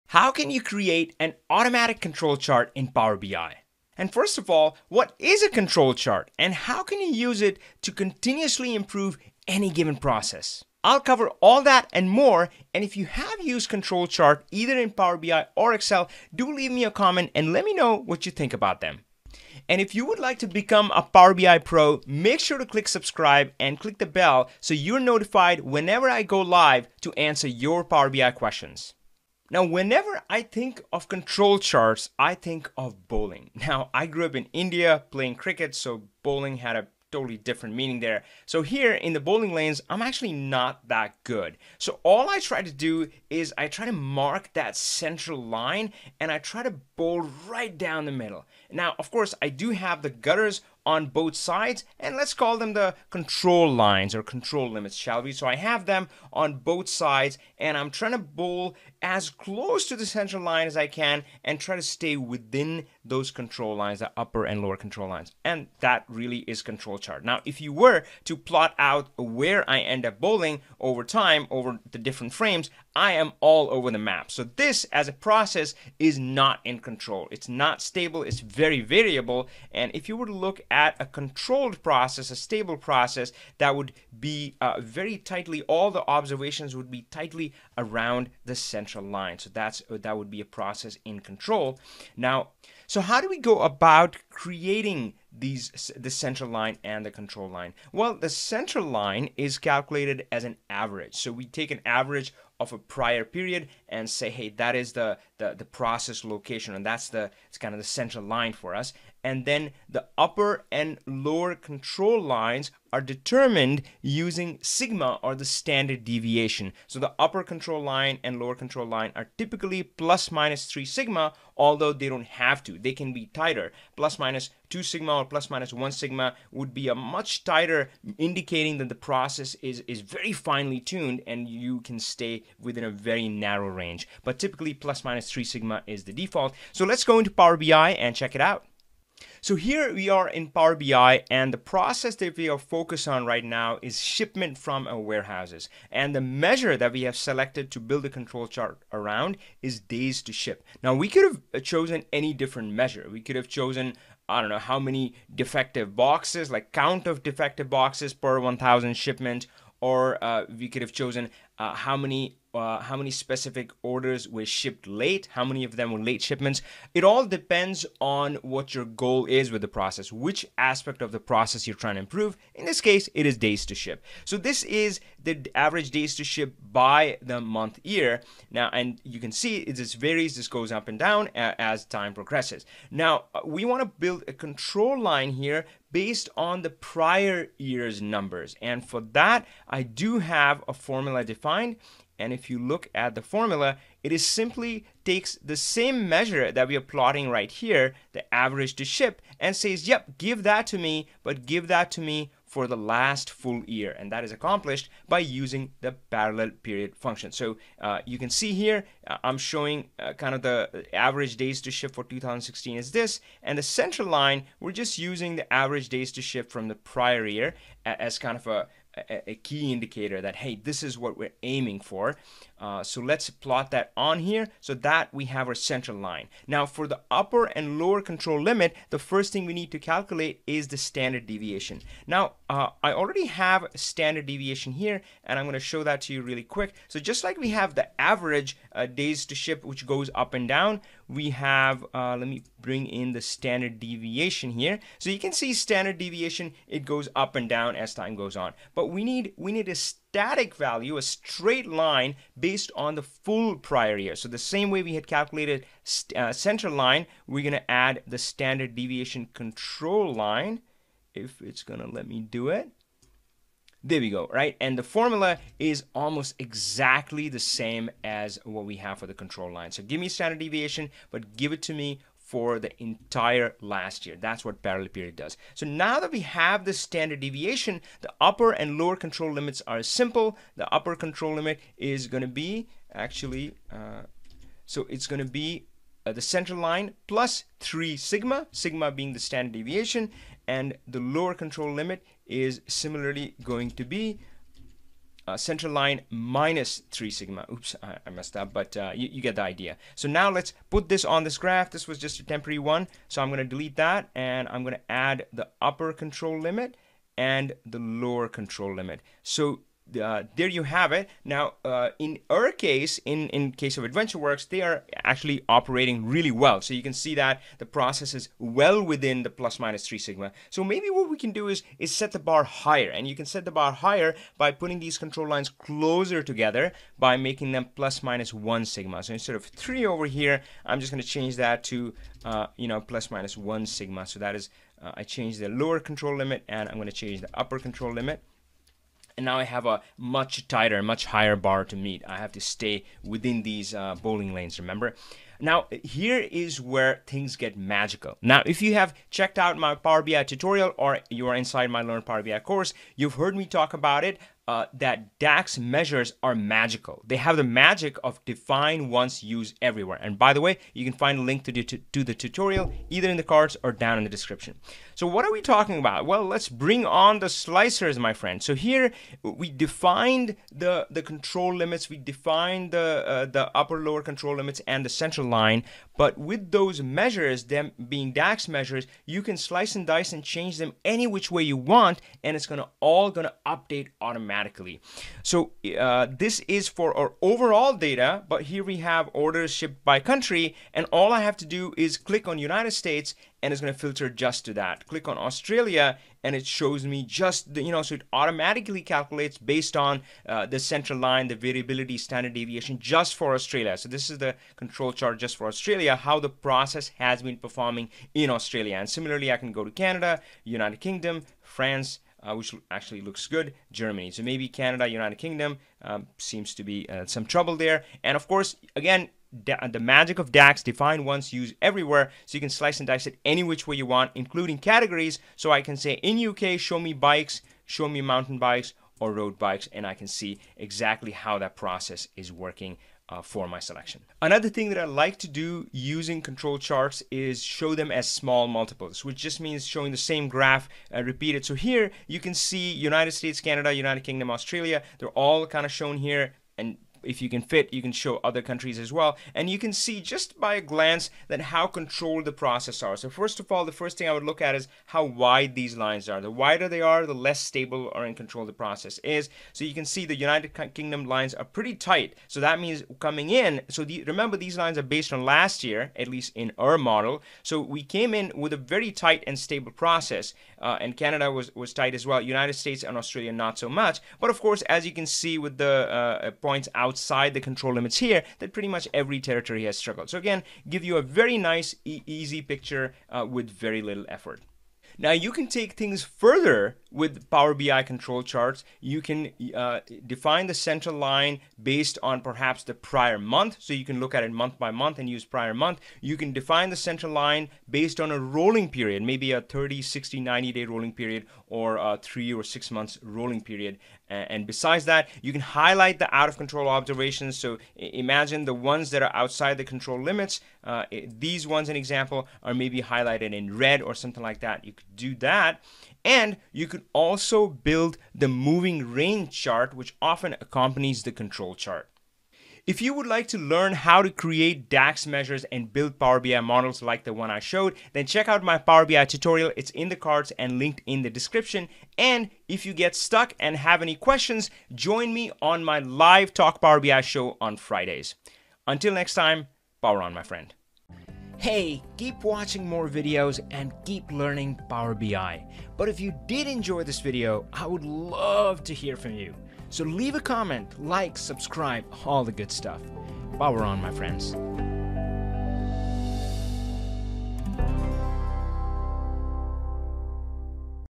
Power BI Control Chart: Create Automatically, Improve Processes Continuously 📈
I'll show you step by step how can you create an automatic control chart in Power BI.
• Control Charts are Statistical Process Control tools used for Continuous Process Improvement in Six Sigma.
• Power BI Control Charts can show the metric along with Upper Control Limit (UCL), Lower Control Limit (LCL) and Mean - all calculated automatically.

⚡Power On!⚡
-Avi Singh